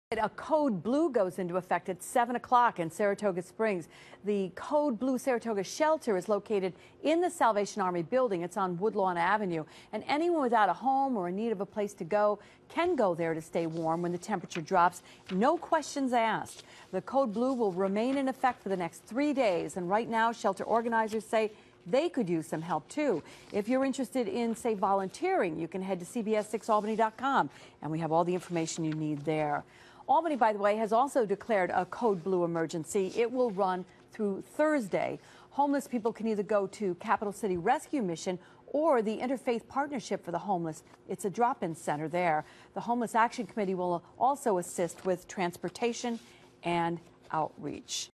Code Blue starts monday for Albany
ALBANY - Officials say they have gone into Code Blue status on Monday through Thursday.  The alert is planned to be lifted Friday morning.

Those in need can go to either the Capital City Rescue Mission at 259 S. Pearl Street or the Interfaith Partnership for the Homeless Drop-In Center at 26 South Swan Street.

Officials say a code blue is called when the temperature is expected to reach ten degrees or less (including the wind chill) or when 12 inches or more of snow is forecasted.